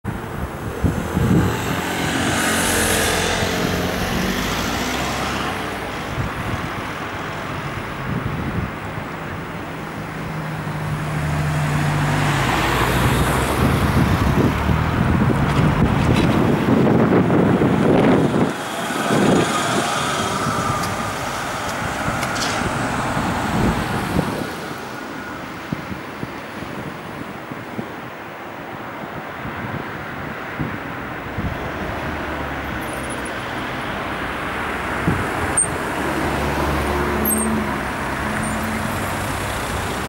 Hess trolleybus te De Laar West rijd de rondonde op lijn 2 en gaan deze straat in
Lijn 2 word tegenwoordig met Trolleys gereden sings een paar jaar!
In De Laar West word lijn 2 lijn 6 en ook andersom (maar 2x Per Uur)